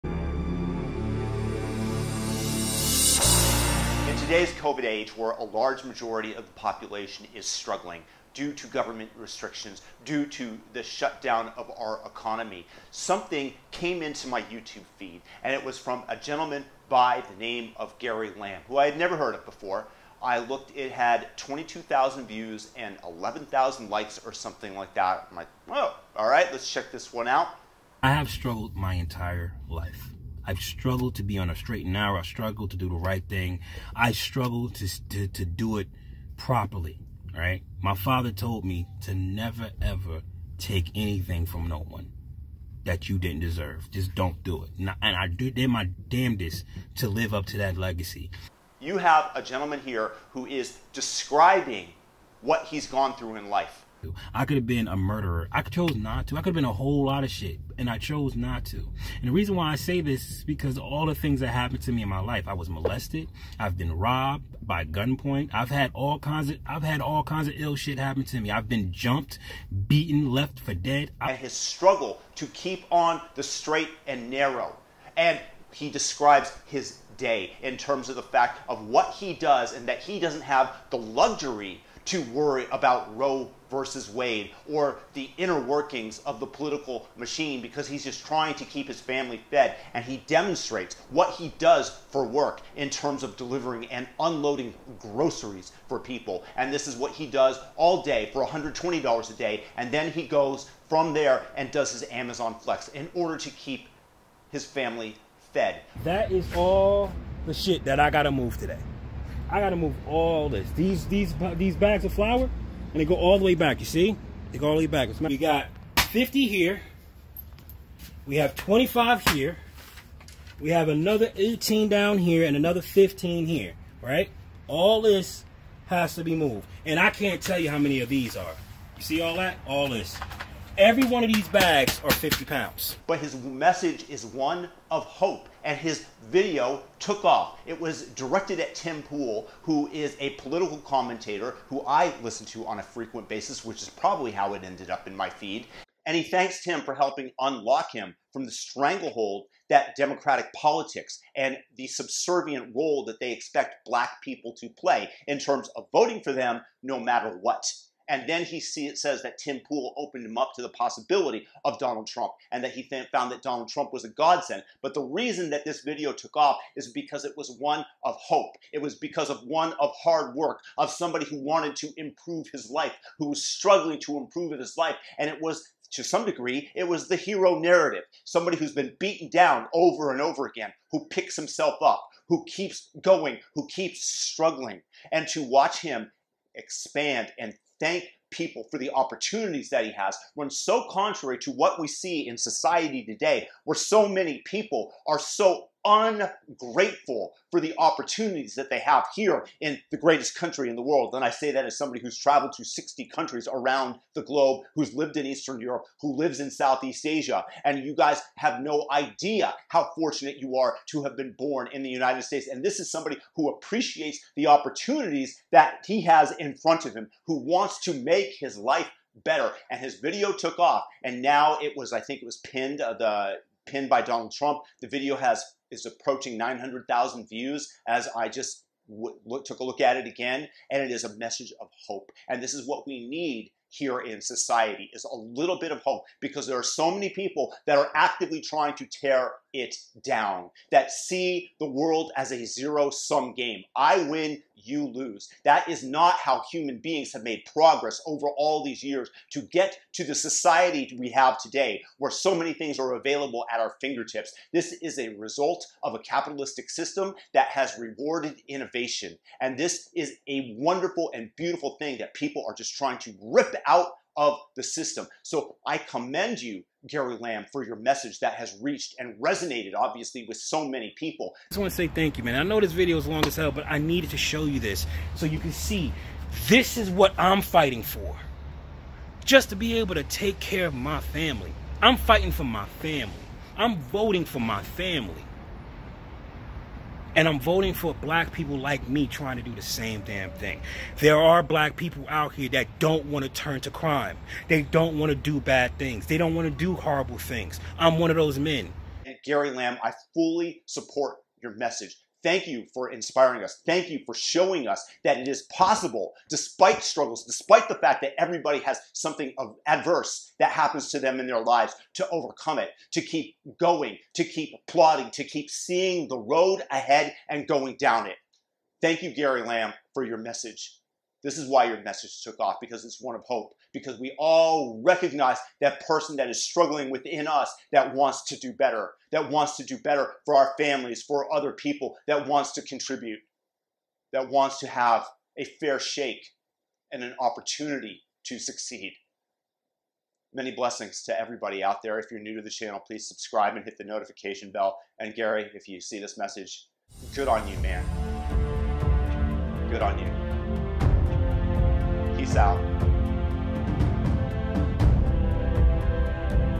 Message of Hope + Perseverance Takes Flight - Gary Lamb's Thank you to Tim Pool
Videos go viral for varying reasons, and in Gary Lamb's video, thank you message to Tim pool, and to Donald Trump for giving him the economic possibilities he needs to succeed, Gary lambs message struck a chord with people around the globe, as they tuned in to his message of hope, perseverance, and persistence in the face of adversity.